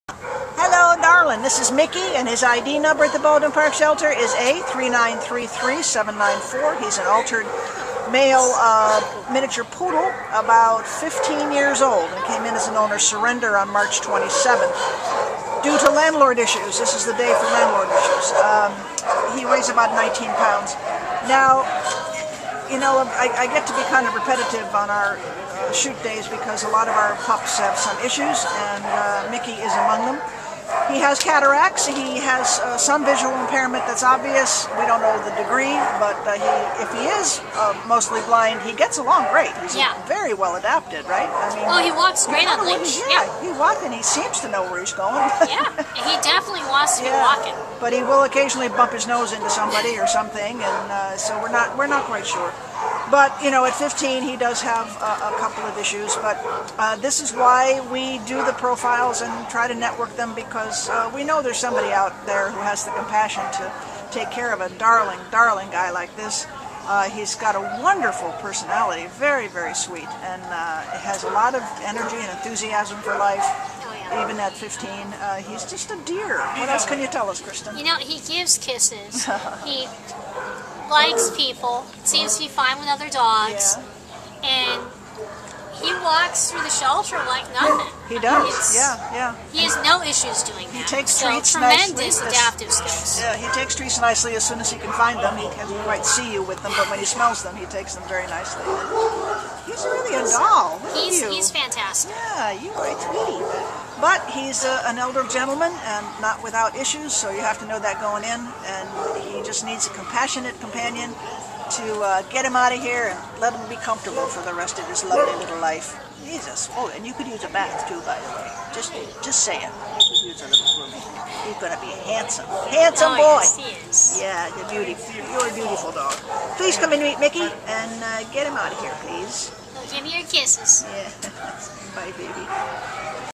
A3933794 Micky | Miniature Poodle
A3933794 Micky is a precious 15-year-old neutered male white-and-apricot Miniature Poodle mix who came to the Baldwin Park Animal Care Center on March 27th as an owner surrender because of landlord problems. Weighing 19 lbs, Micky really needs an angel. The shelter is no place for an elderly gentleman, and he has a number of health issues -- a heart murmur, dental disease, and cataracts. His vision seems to be impaired -- he tends to run into things -- but he has great adaptation skills and it doesn’t slow him down. Micky is spunky and affectionate, good on leash, seems to be housebroken, likes other dogs, and loves to give kisses! Open your home to this senior gem, and he will be the light of your life.